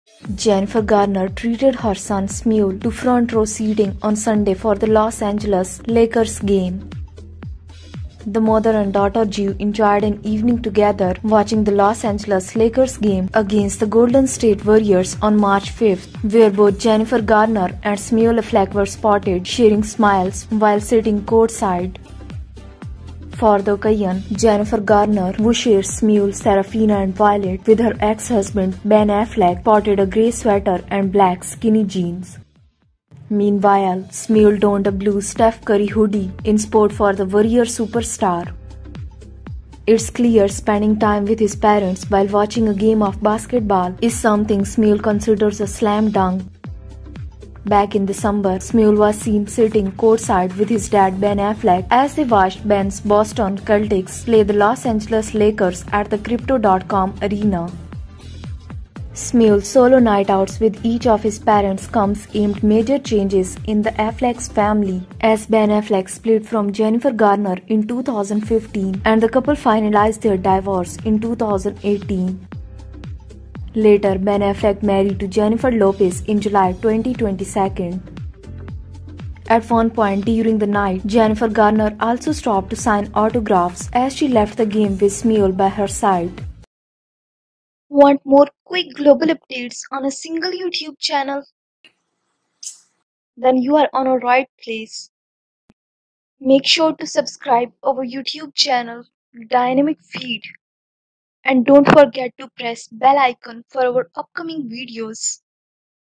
Jennifer Garner treats son Samuel Affleck to front-row seats for Lakers game
Jennifer Garner treats son Samuel Affleck to front-row seats for Los Angeles Lakers game against Warriors.

Jennifer Garner and Son Samuel Affleck Have a Slam Dunk Night Out at Lakers Game.  Jennifer Garner and Samuel enjoyed an evening together watching the Los Angeles Lakers game against the Golden State Warriors on March 5, where they were spotted sharing smiles while sitting courtside.

For the occasion, Jennifer Garner —who shares the Sameul (Sam), Seraphina, and Violet with ex-husband Ben Affleck—sported a gray sweater and black, skinny jeans. Meanwhile, Samuel donned a blue Steph Curry hoodie in support of the Warriors' superstar.

Samuel's solo nights out with each of his parents comes amid major changes in the Affleck family as Ben Affleck and Jennifer Garner split in 2015 before finalizing their divorce in 2018—and later Ben Affleck married to Jennifer Lopez in July 2022.

Watch Jennifer Lopez and Ben Affleck laugh as they watch their own Dunkin' Super Bowl Ad